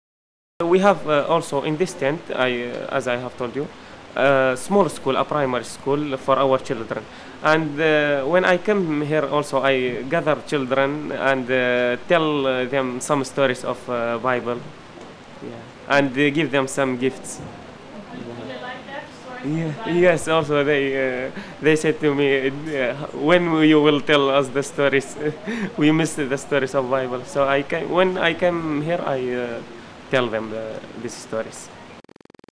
UNHCR tent serves as a church and school in Iraq
Since February 2015 they no longer live in in the partially constructed shopping mall and now live in small trailers that surround the UNHCR tent which assembles as both their ‘church’ and ‘school.’

Since they’ve become displaced by ISIS the UNHCR tent is the only public place Martin’s community has and they still don’t know what has happened to their church in Karamles.

“We don’t hear much about Karamles” Martin shares “but we have our prayers and services in exactly the same way as we did in Karamles, so in this way we remember our church.”